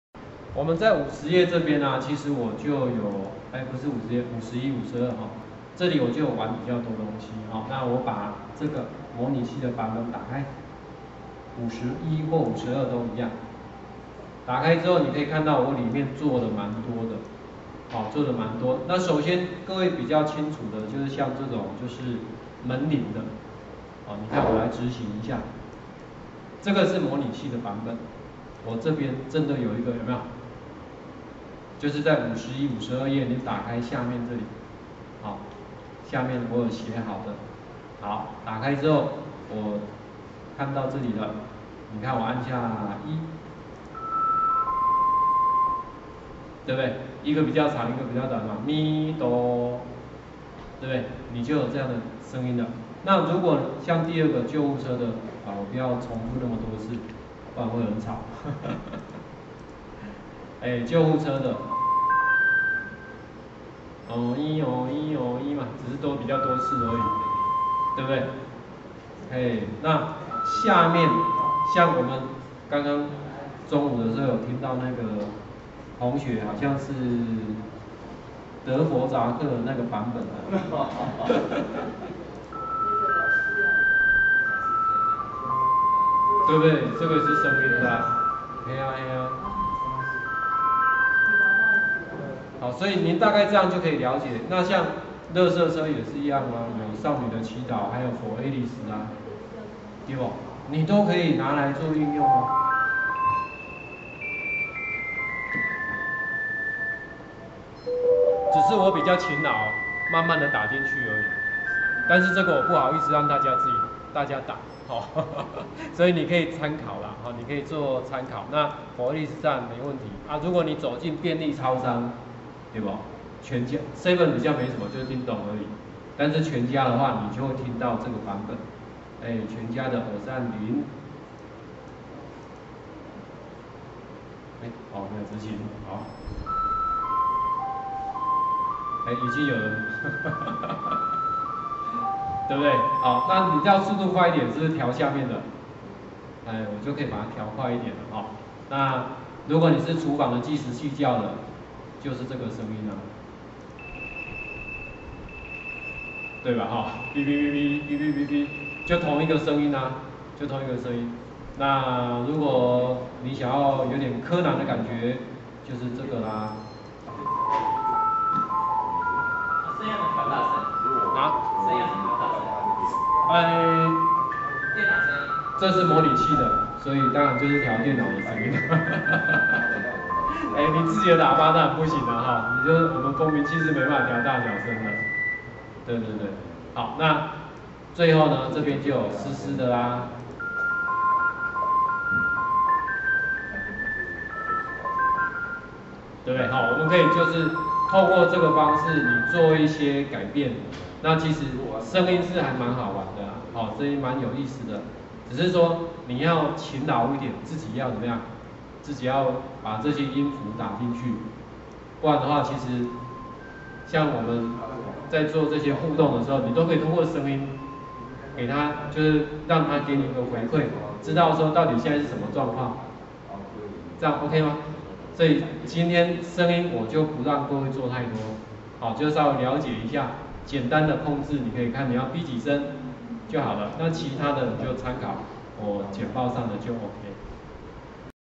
14：用蜂鳴器播放音樂
此影片為「20201106 成功國中：Webduino 智慧小木屋」研習錄影影影片